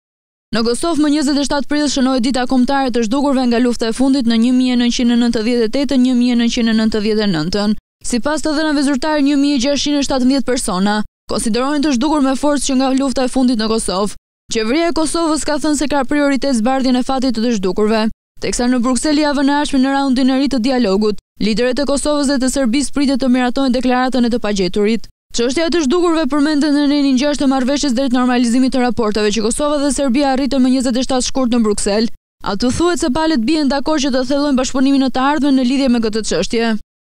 Dita Kombëtare e të Zhdukurve/ Ja çfarë la pas lufta e fundit
Në Kosovë më 27 prill shënohet Dita Kombëtare e të Zhdukurve nga lufta e fundit 1998/99.
Sipas të dhënave zyrtare, 1.617 persona konsiderohen të zhdukur me forcë që nga lufta e fundit në Kosovë.
Qeveria e Kosovës ka thënë se e ka prioritet zbardhjen e fatit të të zhdukurve, teksa në Bruksel javën e ardhshme, në rundin e ri të dialogut, liderët e Kosovës dhe Serbisë pritet të miratojnë Deklaratën për të pagjeturit.
Çështja e të zhdukurve përmendet në nenin 6 të Marrëveshjes drejt normalizimit të raporteve, që Kosova dhe Serbia arritën më 27 shkurt në Bruksel.
Aty thuhet se “palët bien dakord që ta thellojnë bashkëpunimin në të ardhmen”, në lidhje me këtë çështje.

𝐀𝐮𝐭𝐨𝐫 : NTV